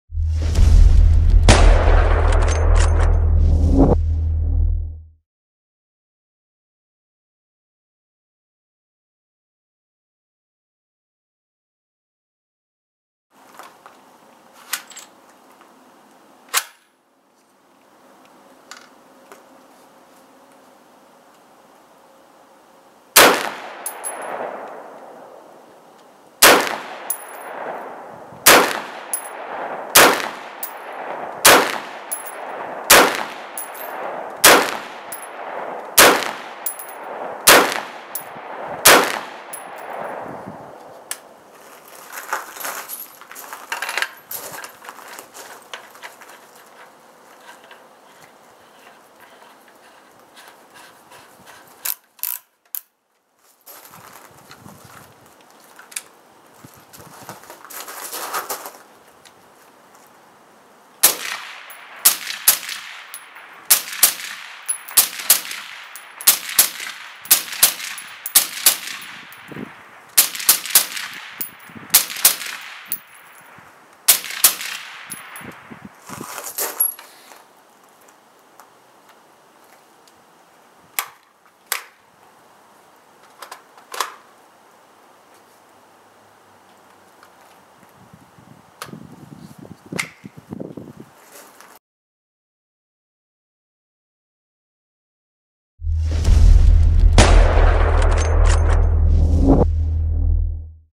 TAVOR
IWI's TAVOR SAR is the perfect answer to a 5.56 SBR without all the NFA paperwork.  And the best part is running suppressed is flawless given it hasn't been chopped and tweaked.